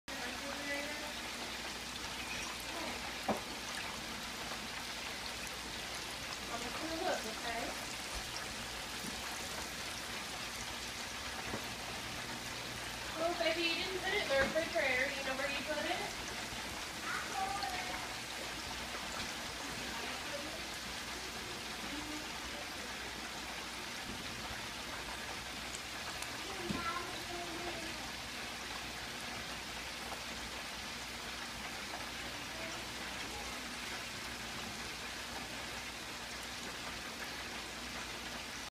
Thanksgiving Fry with Waring Digital Rotisserie Turkey Fryer
Putting my Waring turkey fryer to the test with a massive 18-pound turkey. I wouldn't recommend a bigger bird; it barely fit, even with a tight trussing job. Great little machine.